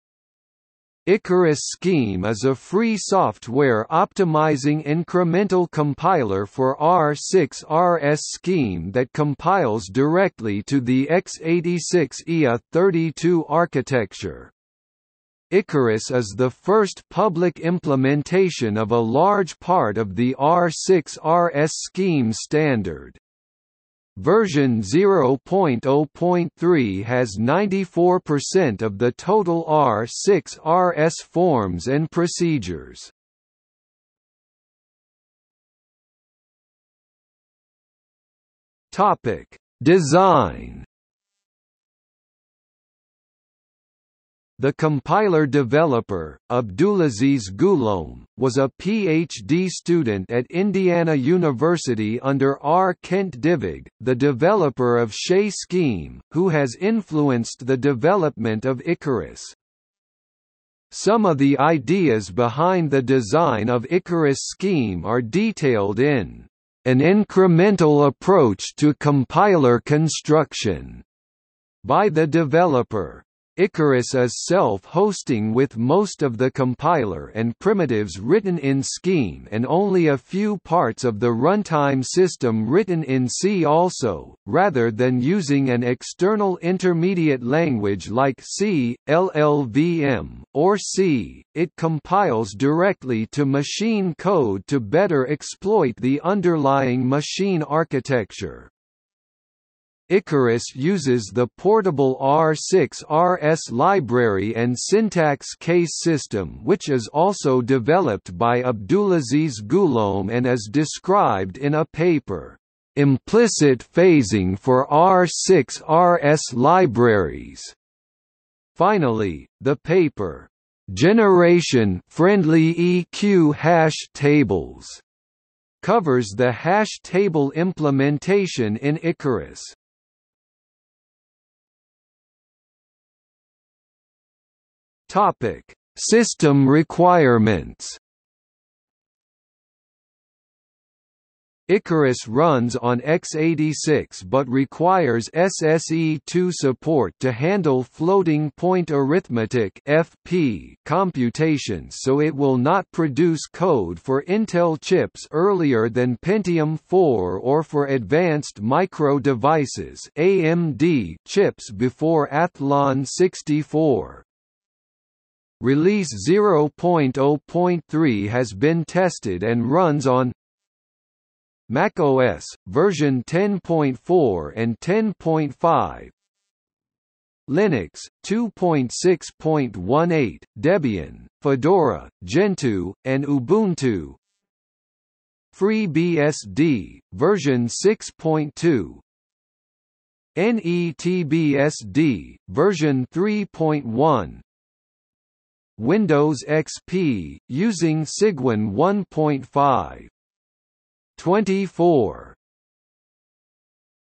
Ikarus (Scheme implementation) | Wikipedia audio article
00:00:35 1 Design
00:02:11 2 System requirements

- Socrates

SUMMARY
Ikarus Scheme is a free software optimizing incremental compiler for R6RS Scheme that compiles directly to the x86 IA-32 architecture. Ikarus is the first public implementation of a large part of the R6RS Scheme standard. Version 0.0.3 has 94% of the total R6RS forms and procedures.